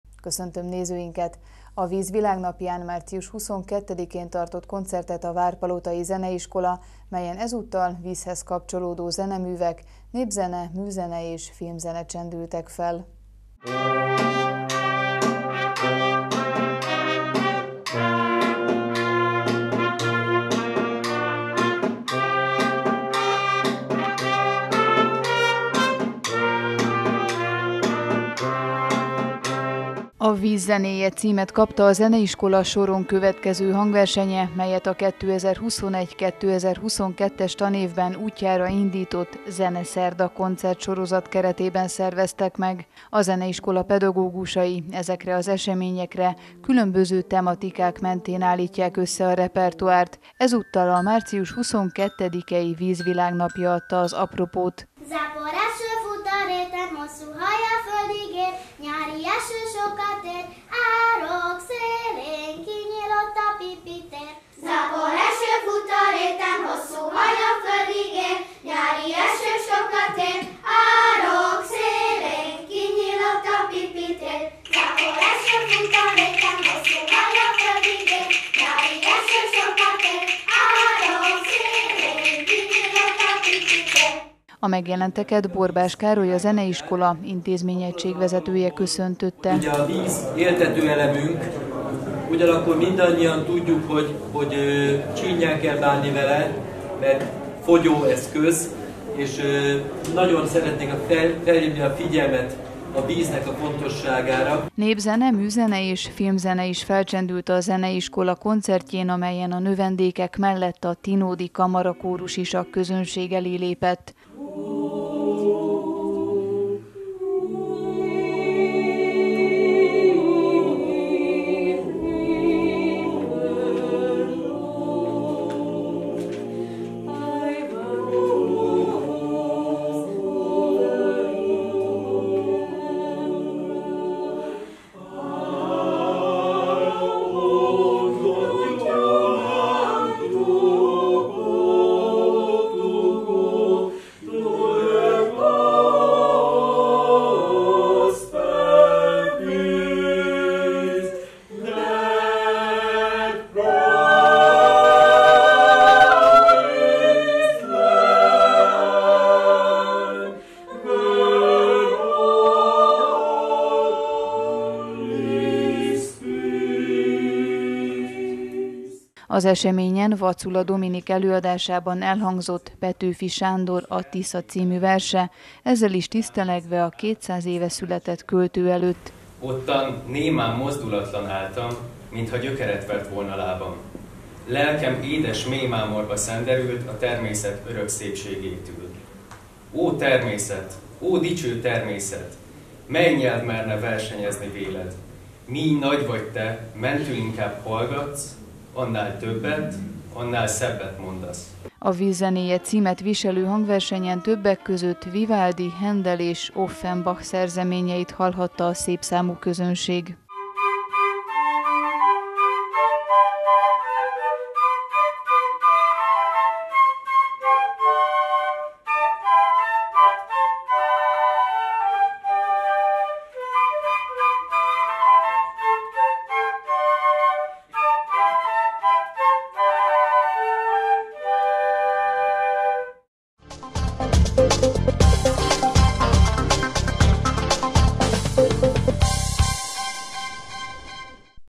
Koncert a viz világnapján / Palotai presszó 2023.03.29.
A víz világnapján, március 22-én tartott koncertet várpalotai zeneiskola, melyen ezúttal a vízhez kapcsolódó zeneművek; népzene, műzene és filmzene csendültek fel.